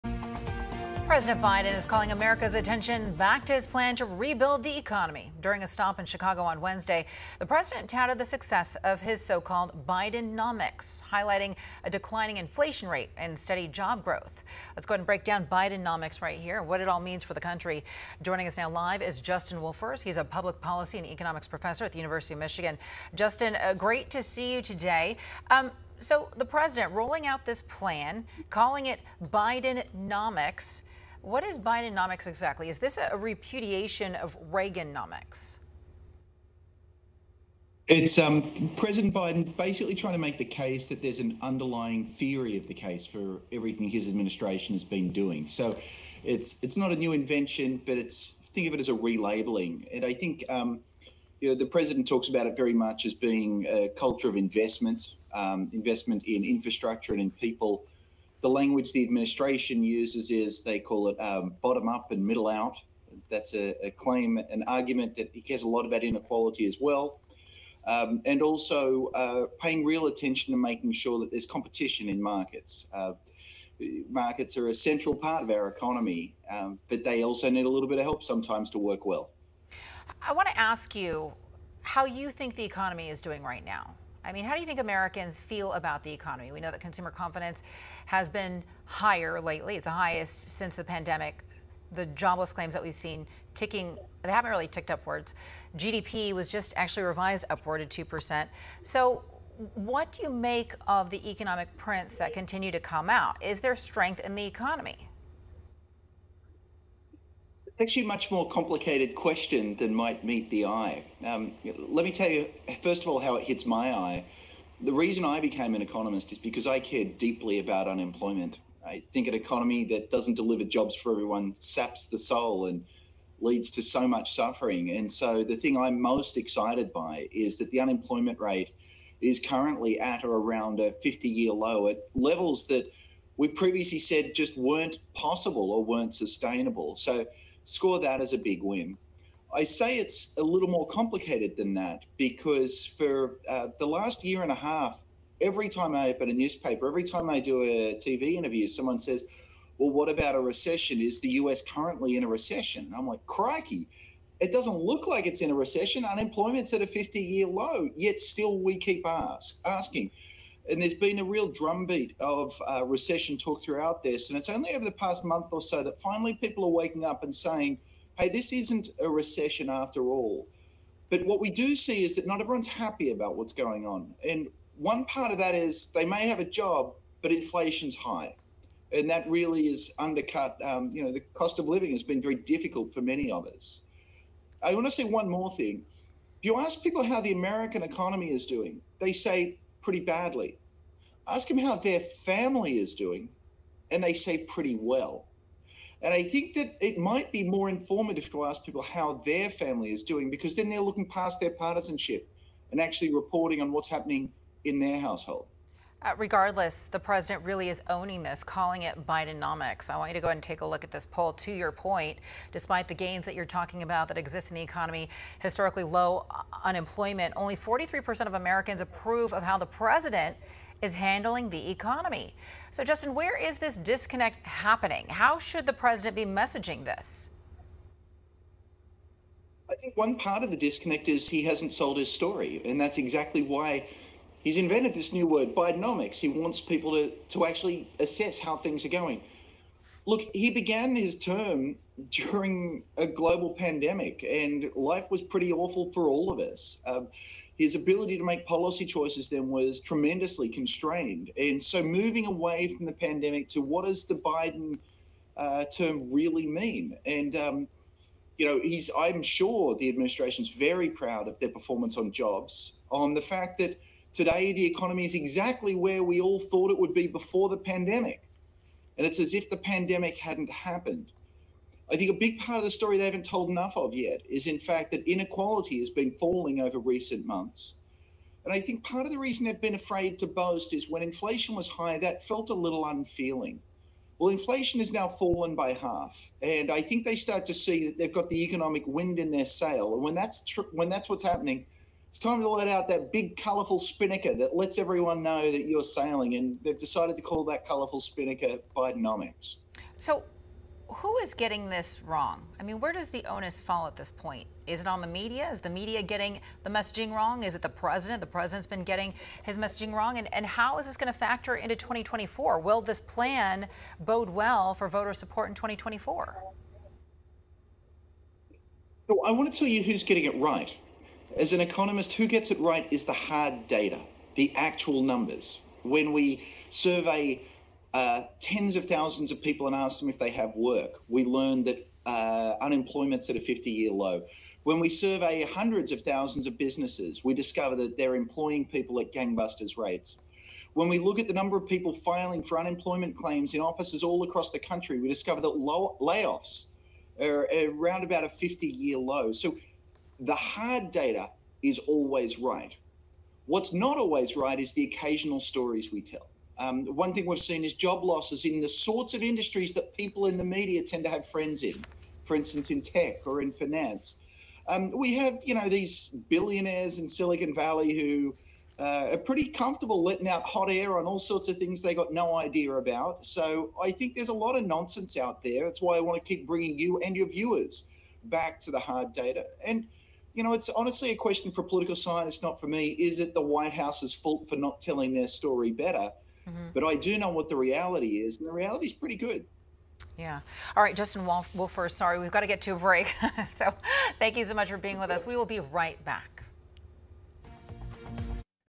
What is Bidenomics? And what is the state of the economy? Justin Wolfers breaks it down
Watch Veronica De La Cruz on Scripps 12 & 3pm eastern scrippsnews.com
Follow Veronica on Youtube, Facebook and Instagram: VeronicaDeLaCruzTV